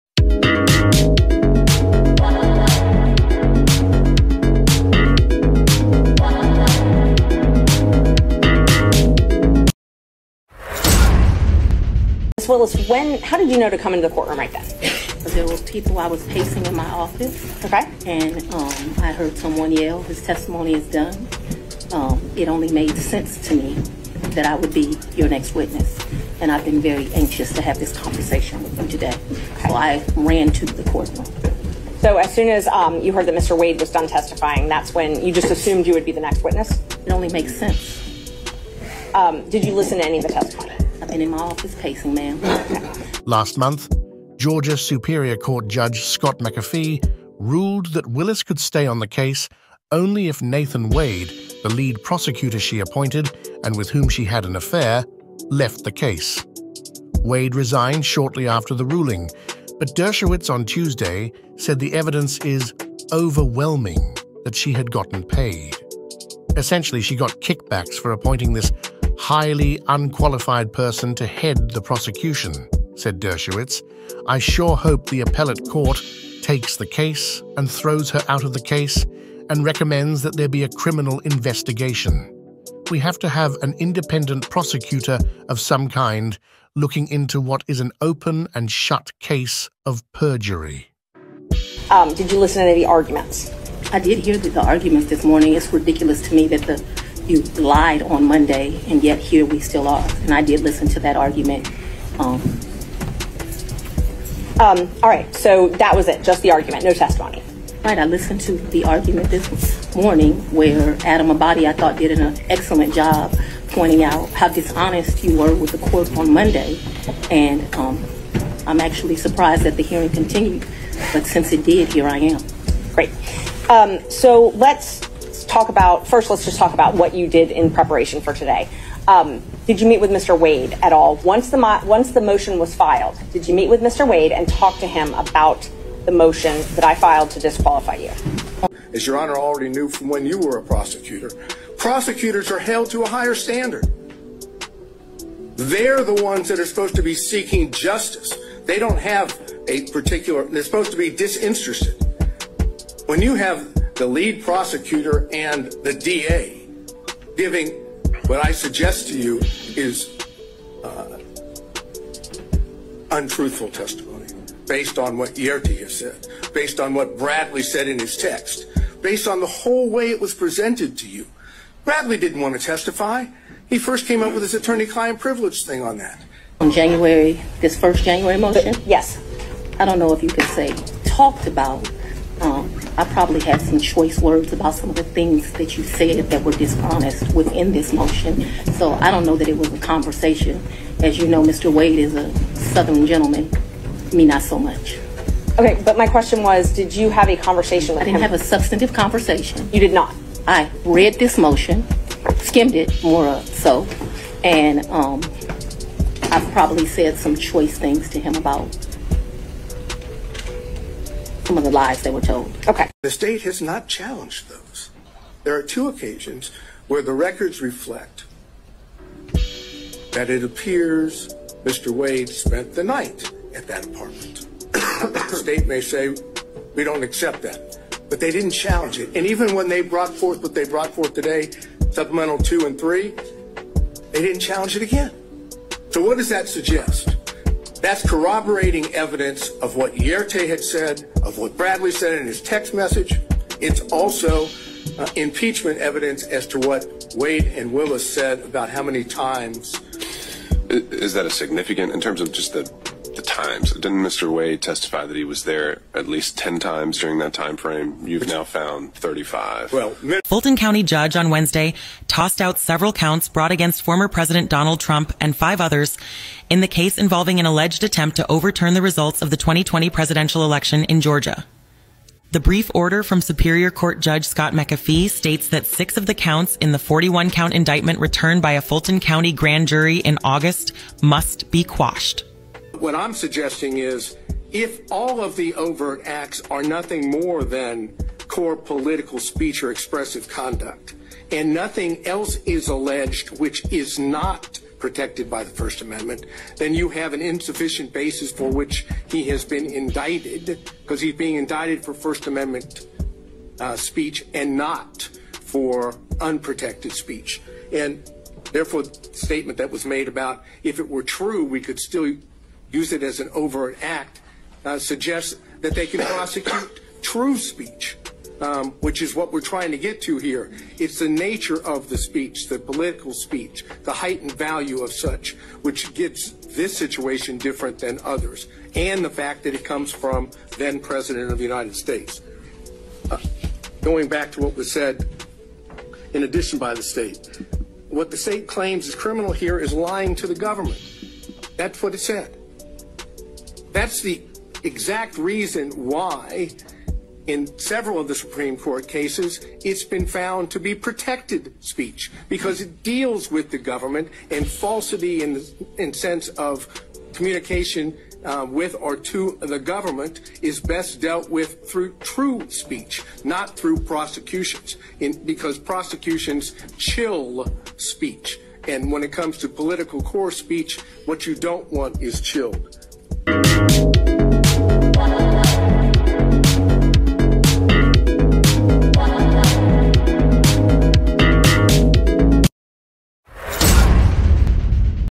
Fani Willis DISQUALIFICATION Saga - FANI's Entire Team REMOVED🚨Trumps Motion of Appeal
Machismo TV

Former Georgia election official has said there were a series of errors in administering the 2020 election in the state, in what may present a challenge for Fani Willis, who is prosecuting Donald Trump over alleged election interference.

In this video, we uncover a compelling courtroom exchange between Miss Willis and the prosecution in a high-profile case. The drama unfolds as Miss Willis takes the stand, revealing shocking details and confronting the accusations against her. The case takes a surprising turn as key evidence is presented, leading to intense courtroom arguments and revelations. Join us as we delve into the legal complexities and ethical dilemmas surrounding this gripping trial.

The case involves former President Donald Trump and others facing indictments related to alleged attempts to overturn the results of the 2020 presidential election in Georgia. The legal proceedings take a controversial twist as Judge Scott McAfee makes crucial decisions regarding the charges brought forth. The video explores the First Amendment implications, political speech protections, and the challenges of prosecuting true speech in a political context.

Witness the heated exchanges, legal analysis, and the quest for justice in this captivating courtroom drama.

Don't miss out on this engaging and thought-provoking video that sheds light on the intricacies of the legal system and the pursuit of truth in the face of adversity.